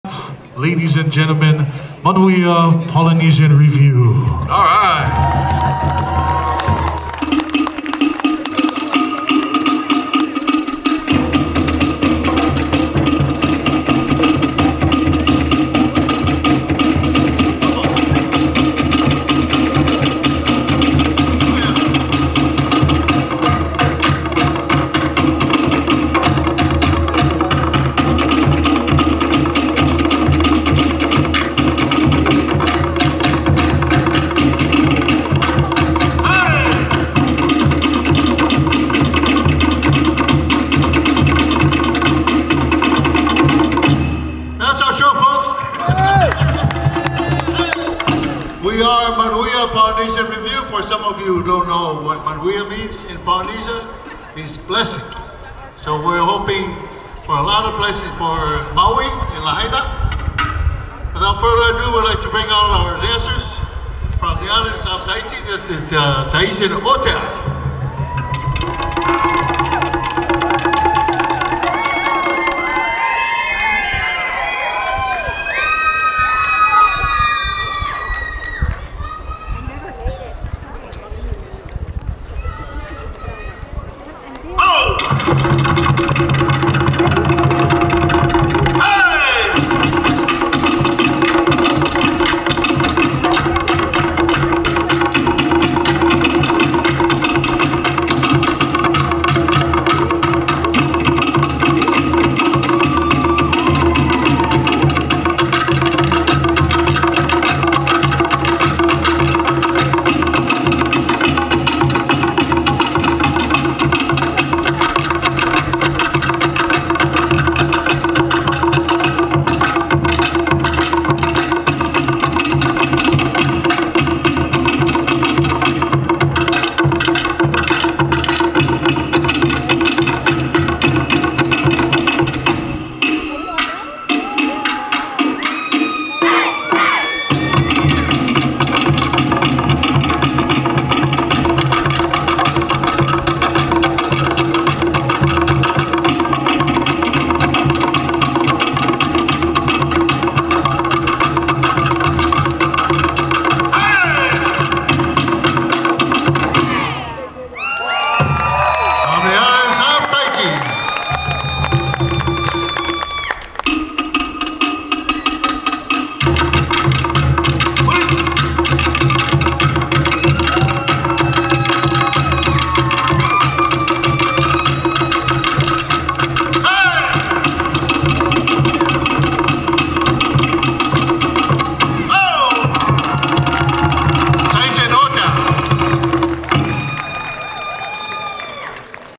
Manuia Polynesian Review | 2023 Bay Area Aloha Festival | August 12
Manuia Polynesian Review at the 2023 Bay Area Aloha Festival at San Mateo County Event Center.